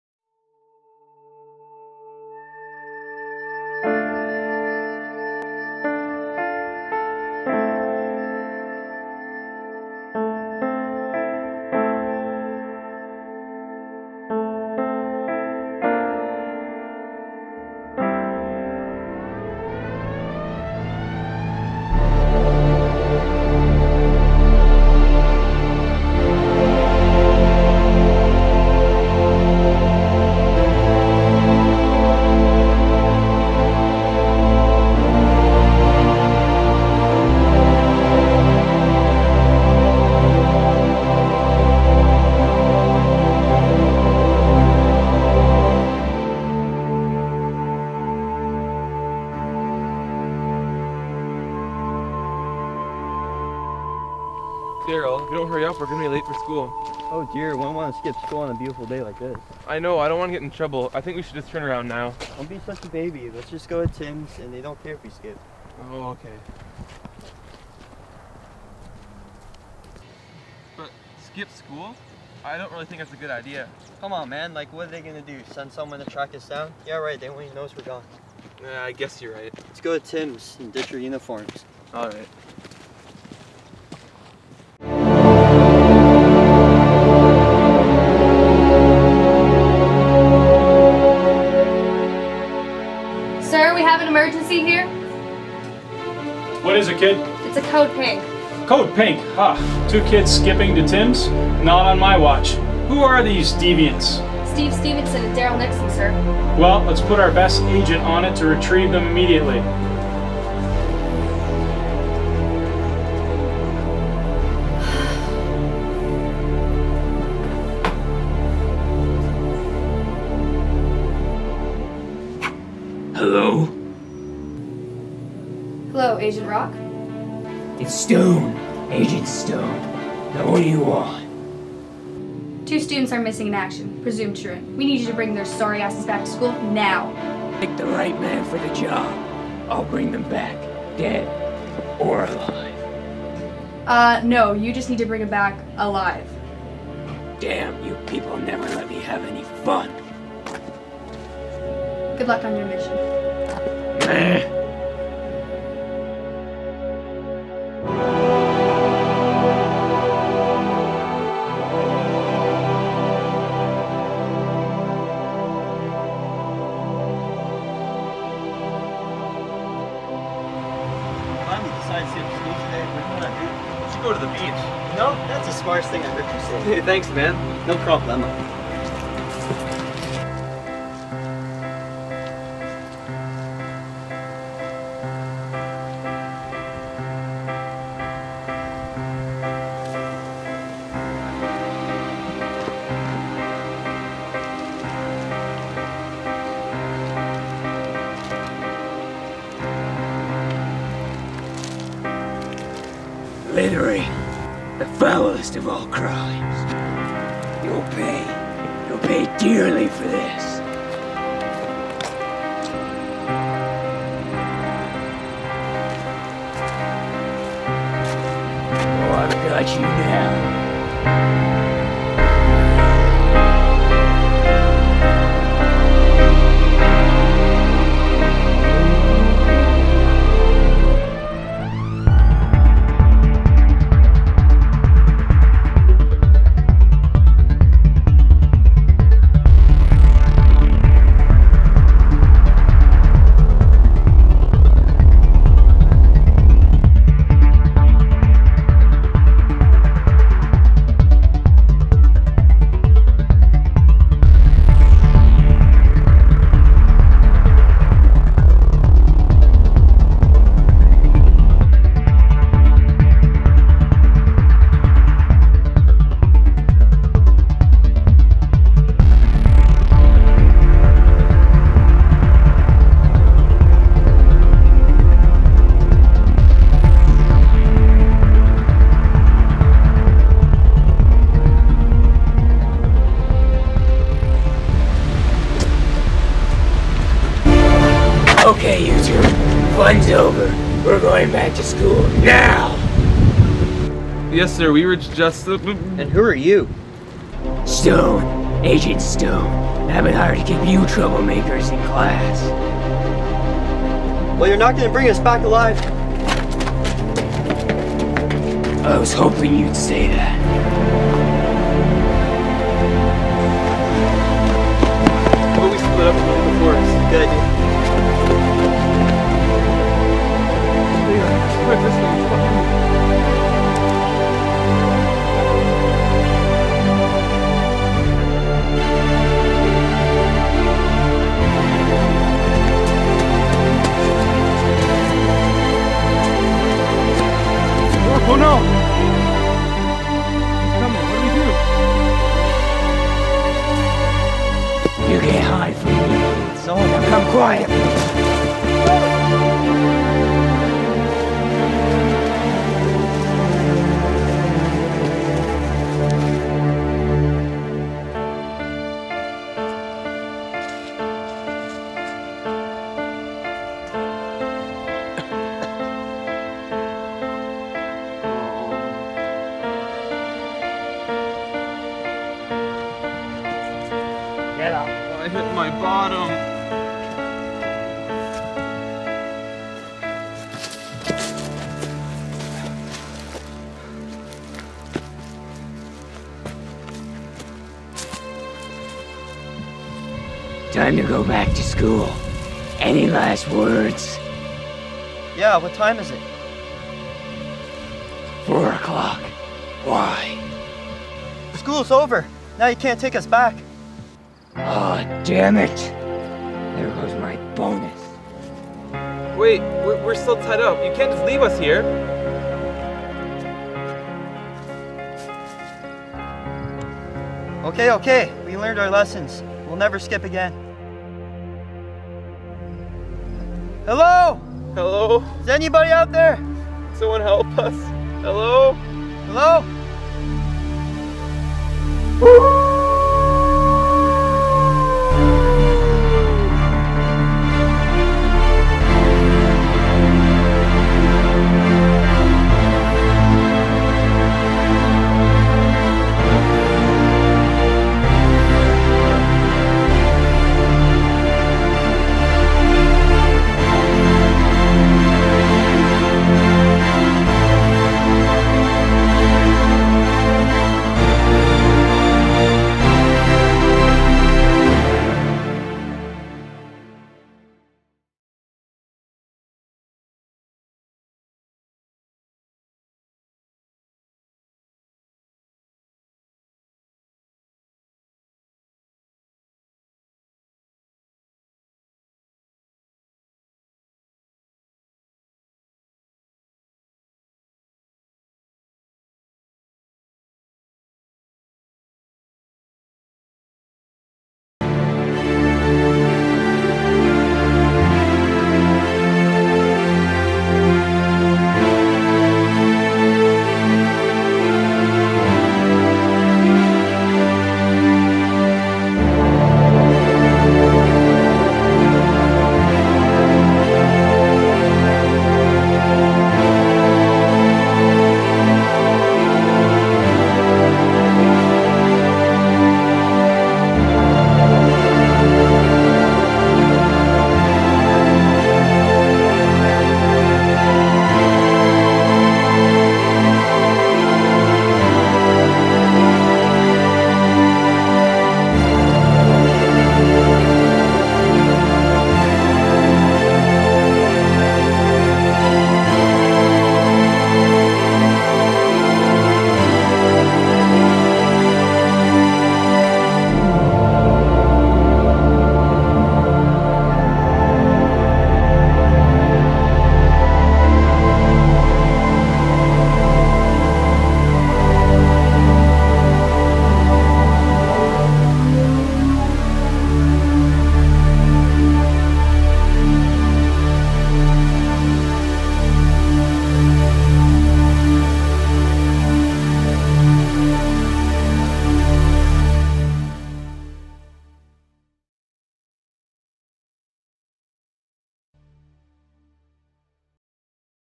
Skip Tracer - Michael F.
A short film completed as part of his TGJ4M Communications Technology course.